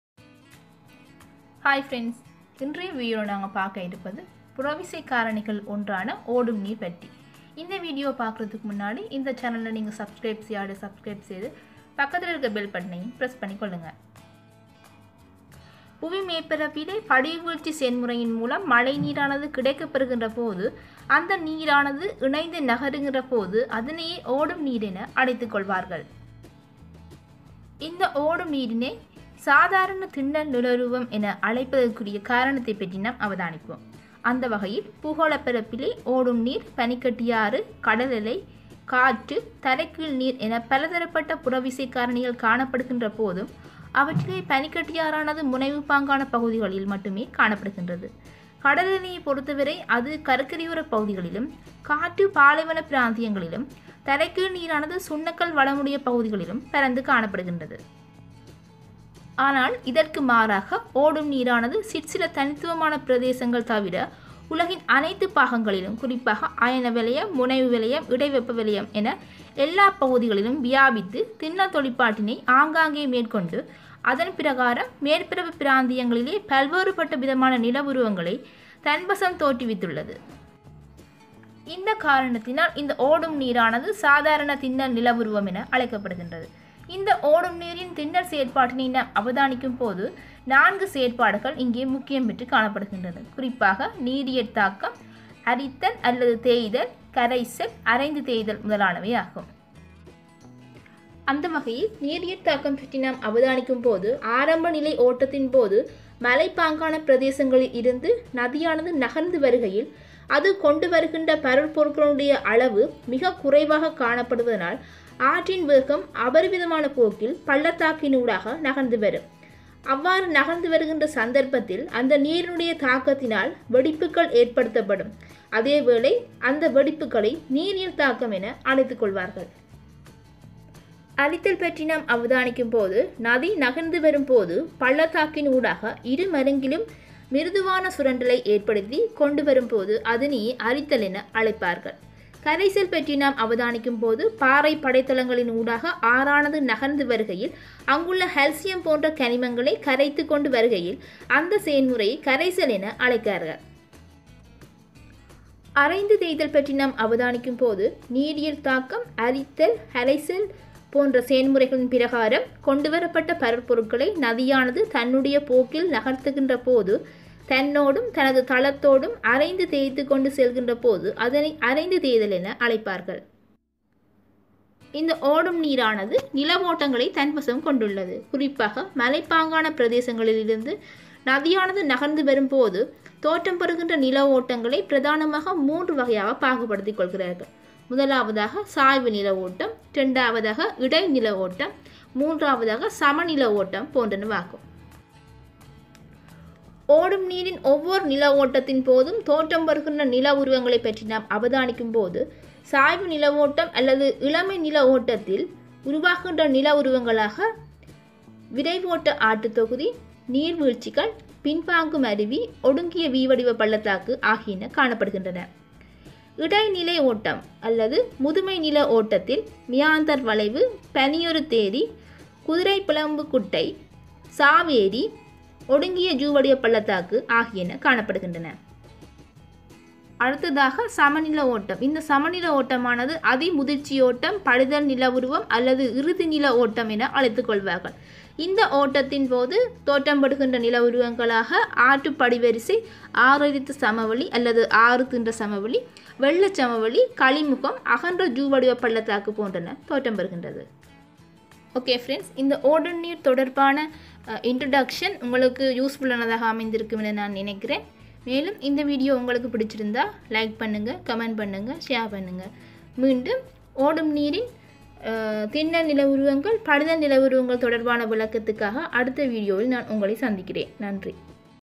ஓடும்நீரின் செயன்முறை |Introduction To River landforms In Tamil | Tamil Geography News
Introduction To River landforms In Tamil

This video is explain to introduction to river landforms. Erosional landforms are  V-shaped valleys, interlocking spurs, waterfalls and gorges. Meanders and oxbow lakes are formed from erosion and deposition. Depositional landforms include floodplains.

Thanks.